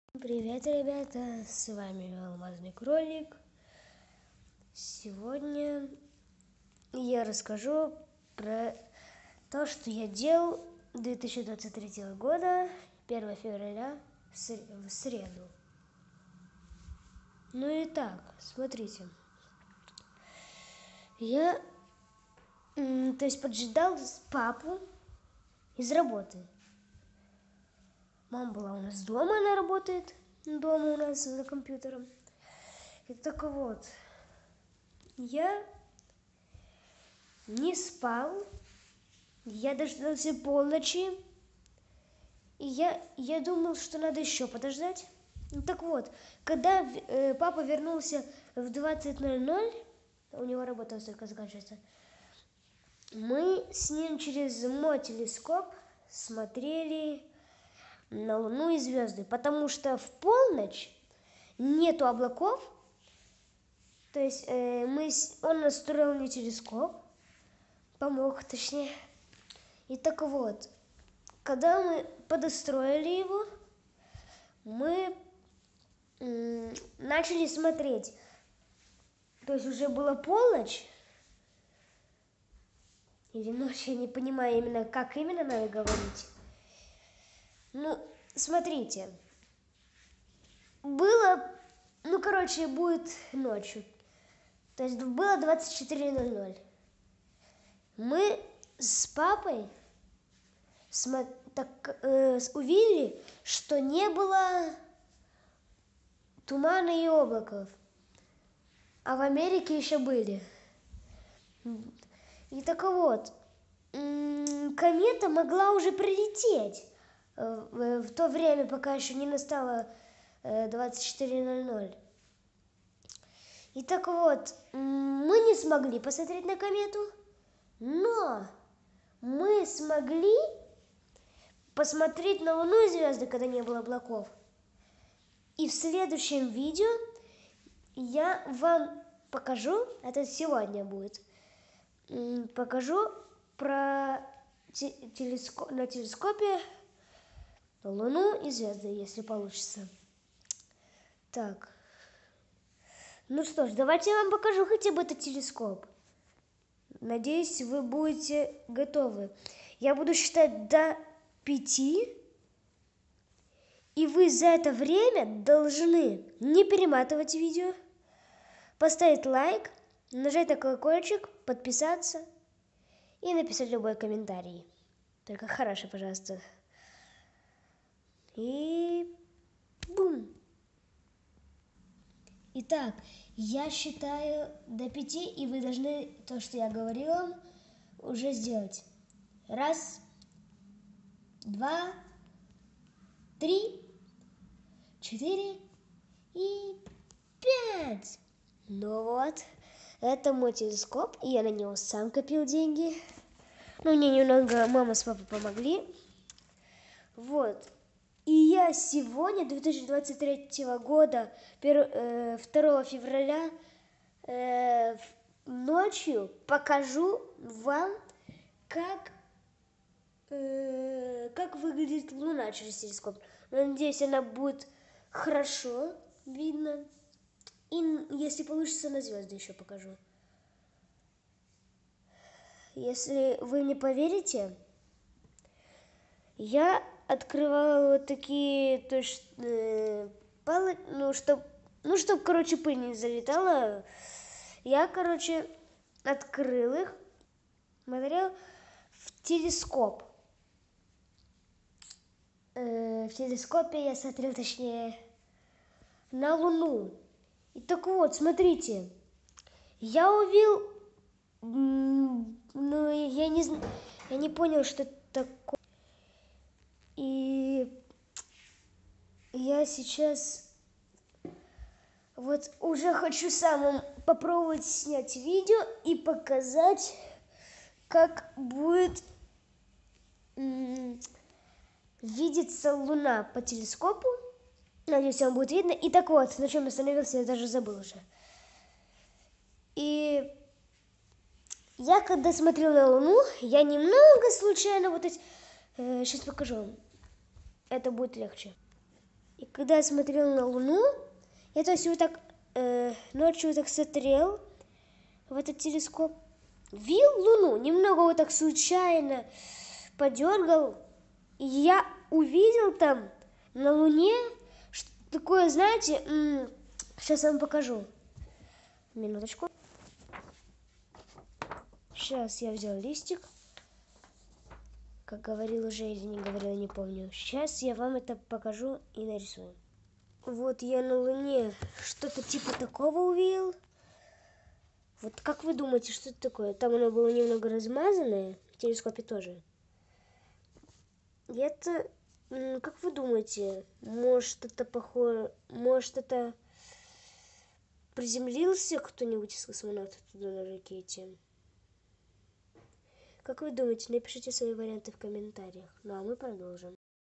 новости 02.02.2023г.
в данном видео я покажу ночью луну и звёзды 🌚⭐⭐